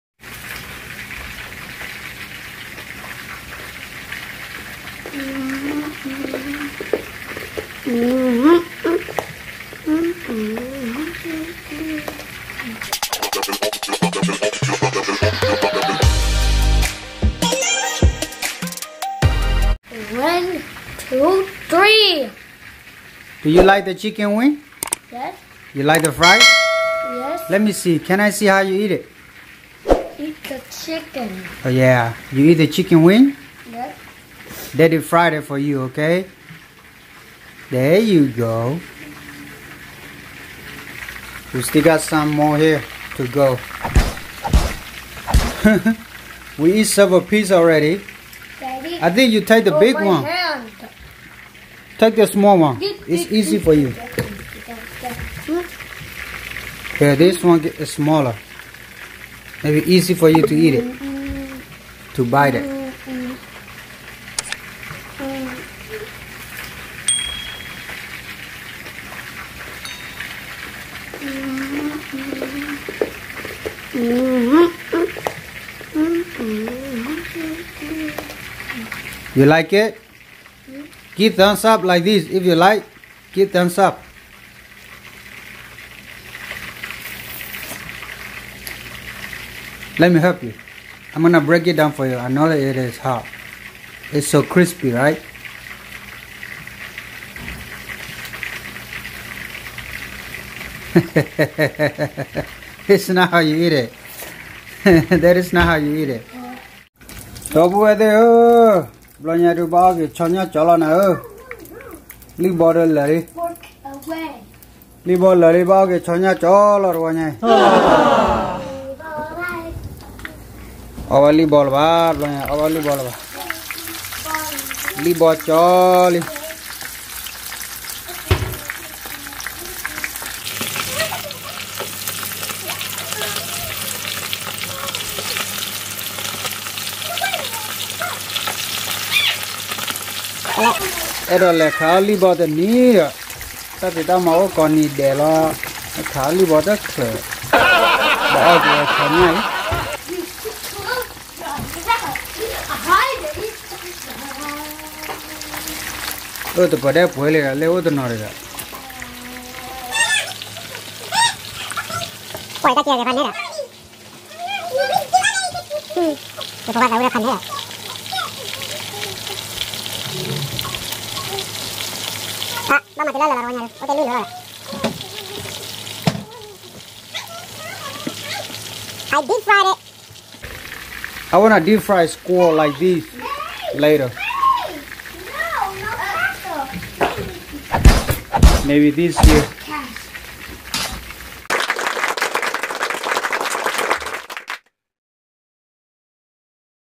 How to deep fried chicken, Easy Fried Chicken Wing.
Today we are just fried chicken wing for lunch. i will make a better video soon. Thanks for your time and hope you enjoy the video.